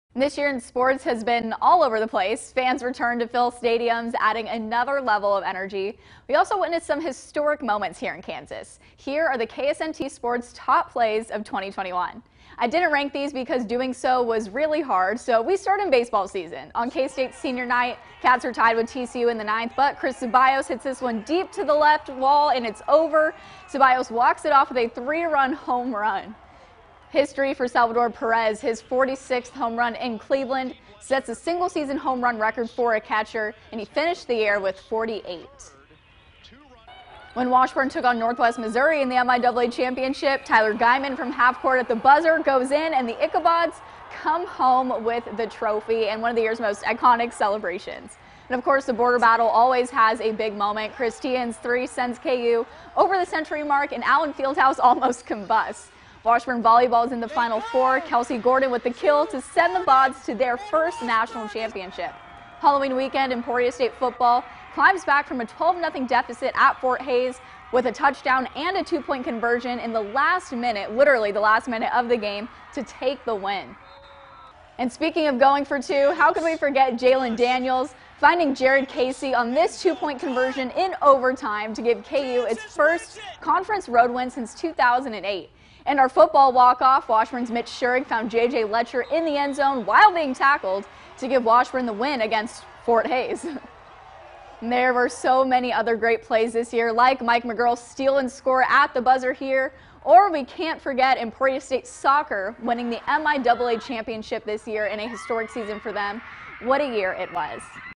2021 Plays of the Year
We witnessed some historic sports moments in Northeast Kansas. Here are the KSNT Sports top plays of 2021.